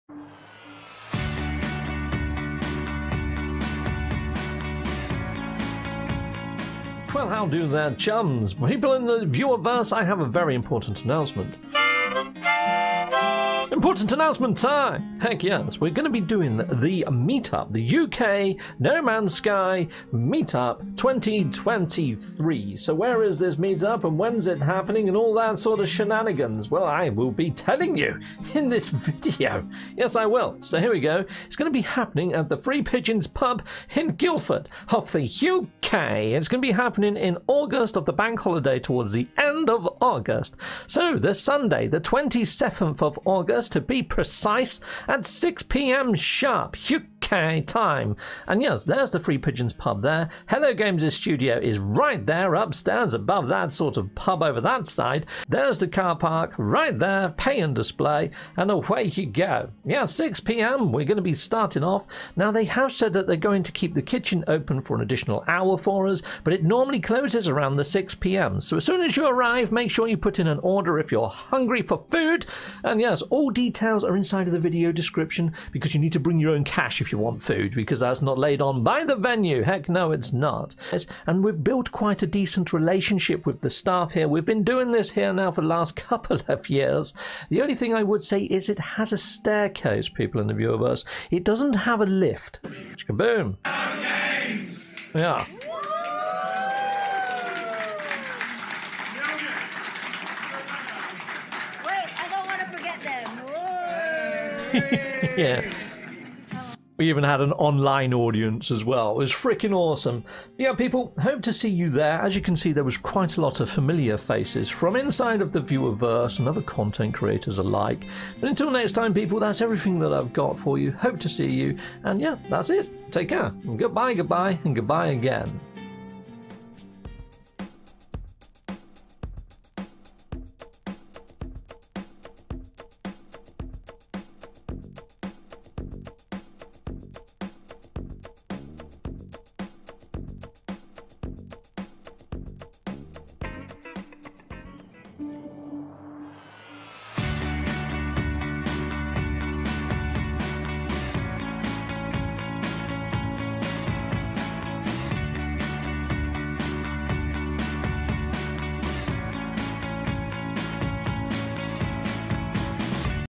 No Man's Sky UK Meetup 2023 Trailer Video Guildford Three Pigeons 6pm GMT 27th Aug
No Man's Sky 2023 7th Anniversary Guildford Three Pidgeon's Pub Meetup NMS Event Sunday 27th August UK 6pm at The Three Pigeons - 169 High St, Guildford GU1 3AJ - Up Stairs.
(There is no lift, and you can see the stair case in the video, sadly it's not suitable for all).

Rough Schedule
6 to 8pm - Food and Mingle

8 to 8.45pm - Raffle Draw

9 to10pm - Quiz
10 to Close - Merry Mingle

Parking: After 6pm the Highstreet is said to be free parking - Check as things change !!

Food
Service options: Dine-in · Kitchen Closes normally around 7pm they may keep it open to 8pm (Order food when you arrive).
(Food and drink can be purchased at the venue, not supplied by the event organisers, bring your own money).

Raffle
Free raffle tickets given when you arrive, hoping to have enough items that everyone will get something as a keep-sake.

Quiz
Quiz is also free to enter, may have prizes for the table/group that wins.
(Could just be bragging rights - will wait and see)

Mingle
Money I get from Super Chats and Patreon for the two months prior I will put behind the bar, so there may be free drinks (limited obviously) for either the 1st or last hour, unsure yet what I am doing in that regard... May just buy rounds of shots... not sure...

Hope to see as many of you attending as possible.
Please add a comment to let us know if you are coming for Sure.
(To give us a rough number of people attending).

There are many local Hotels.
We tend to use: Travel Lodge / Premier Inn / Harbour Hotel
(All are a fairly close walk or a quick taxi ride)